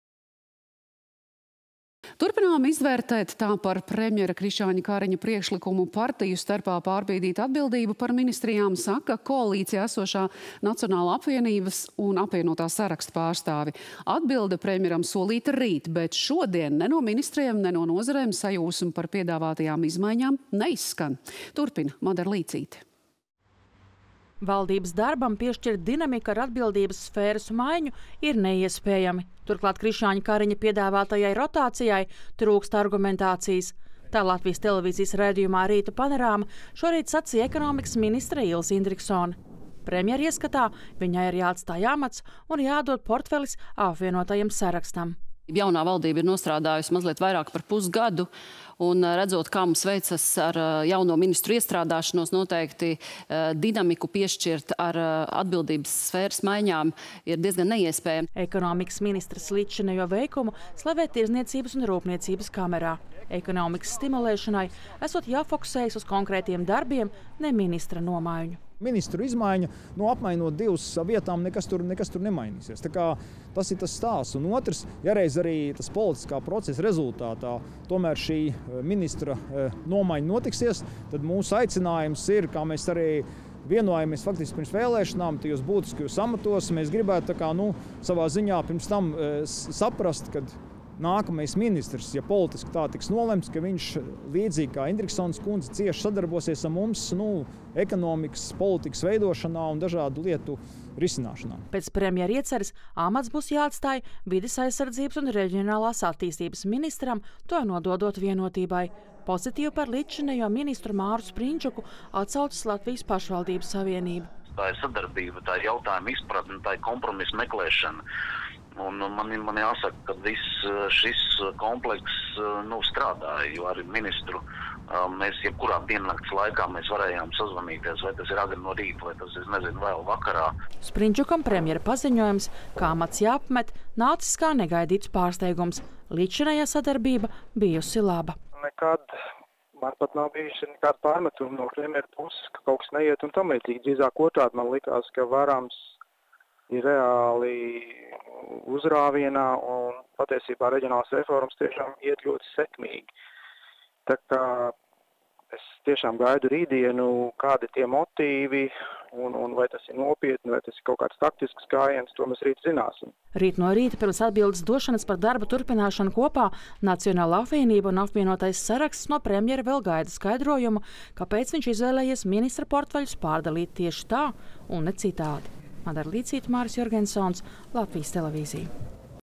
Vērtē premjera piedāvājumu, pagaidām loģiku neredz
Turpinām izvērtēt – tā par premjera Krišjāņa Kariņa priekšlikumu pārbīdīt ministrijas starp partijām saka koalīcijā esošā “Nacionālā apvienība” un “Apvienotā saraksta” pārstāvji.  Atbilde premjeram solīta rīt, bet šodien ne no ministriem, ne no nozarēm sajūsma par piedāvātajām izmaiņām neizskan. Turpina Madara Līcīte